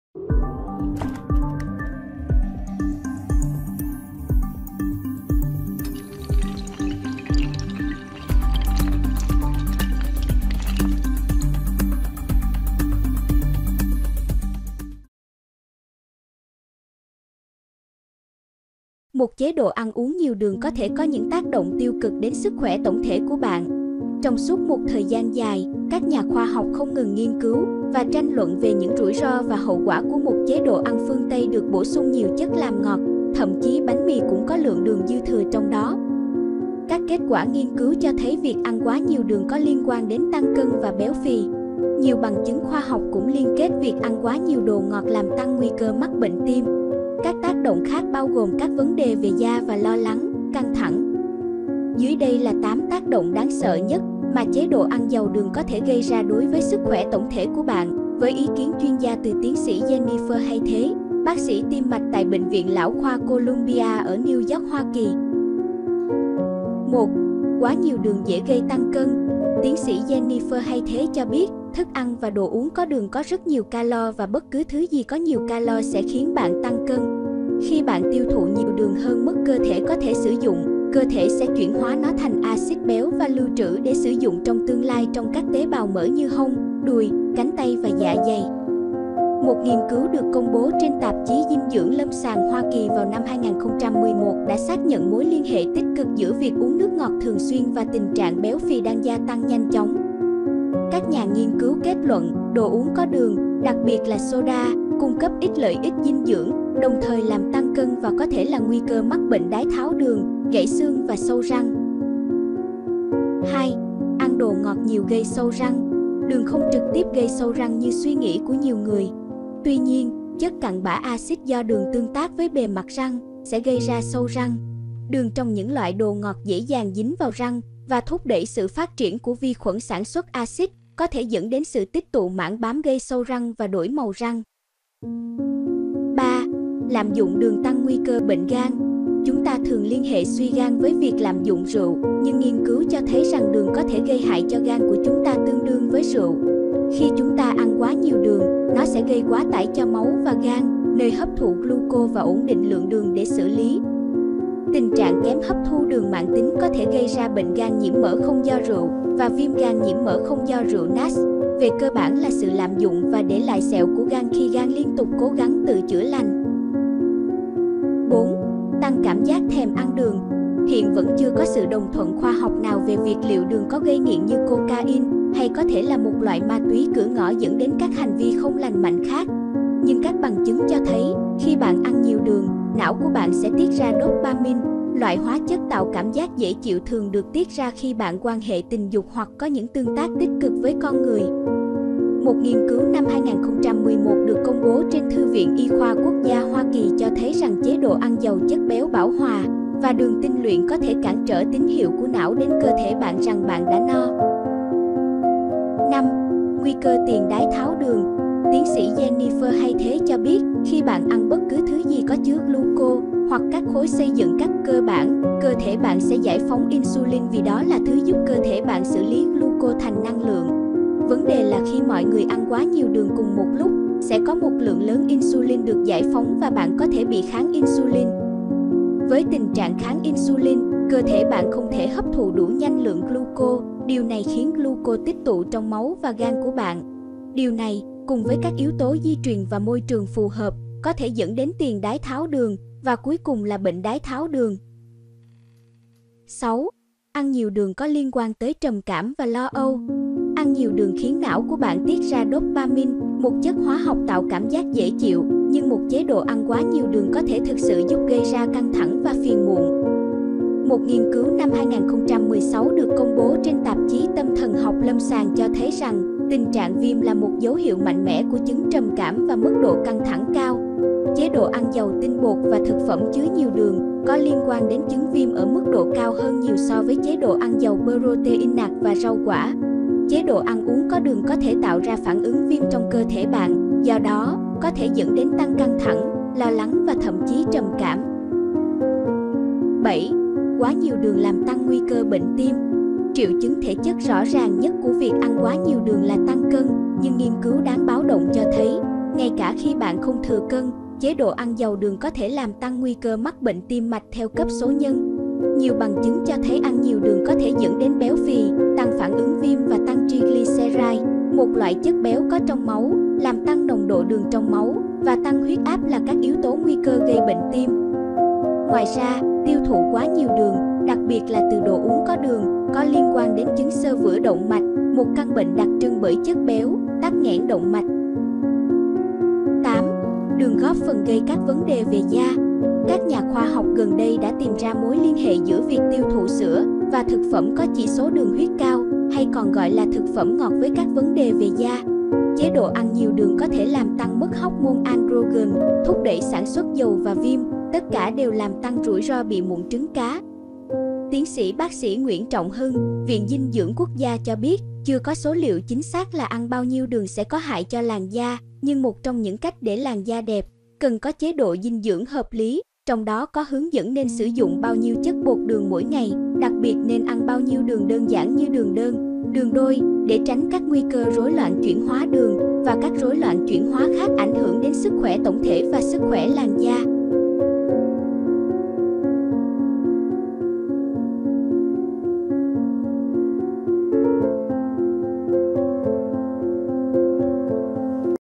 8 điều đáng sợ xảy ra với cơ thể, tăng nguy cơ bệnh khi bạn ăn quá nhiều đường || Sức khỏe mỗi ngày
Trong suốt một thời gian dài, các nhà khoa học không ngừng nghiên cứu và tranh luận về những rủi ro và hậu quả của một "chế độ ăn phương Tây" được bổ sung nhiều chất làm ngọt (thậm chí bánh mì cũng có lượng đường dư thừa trong đó).

Các kết quả nghiên cứu cho thấy việc ăn quá nhiều đường có liên quan đến tăng cân và béo phì. Nhiều bằng chứng khoa học cũng liên kết việc ăn quá nhiều đồ ngọt làm tăng nguy cơ mắc bệnh tim. Các tác động khác bao gồm các vấn đề về da và lo lắng, căng thẳng.

‎Dưới đây là 8 tác động đáng sợ nhất mà chế độ ăn giàu đường có thể gây ra đối với sức khỏe tổng thể của bạn với ý kiến chuyên gia từ Tiến sĩ Jennifer Haythe, bác sĩ tim mạch tại Bệnh viện Lão khoa Columbia ở New York (Hoa Kỳ).